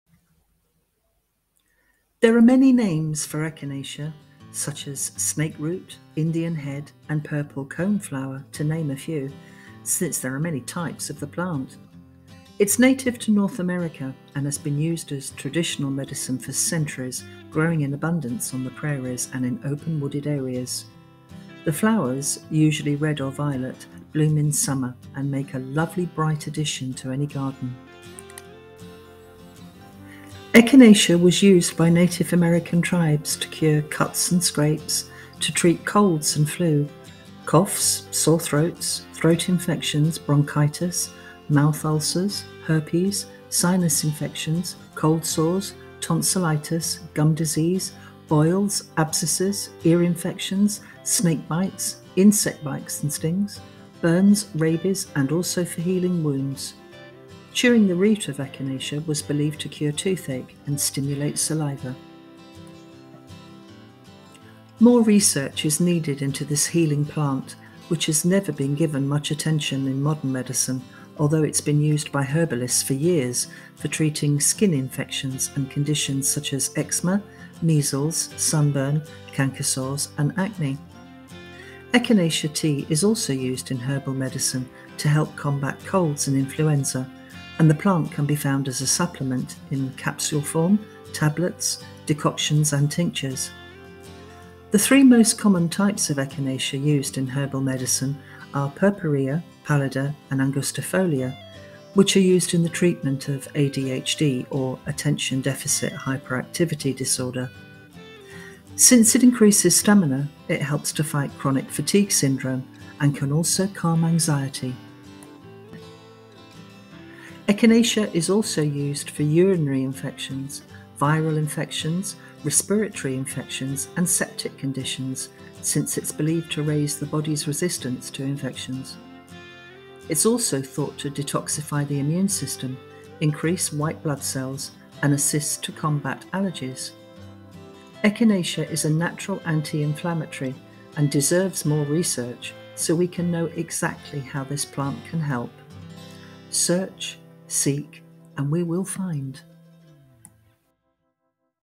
Echinacea - Search, Seek and Find
Echinacea has many names since there are many different types of the plant. It has multiple uses as a medicinal plant and is a popular choice for herbalists. It has never been given much attention in modern medicine and more research needs to be done so we can see the full potential of what Echinacea can do.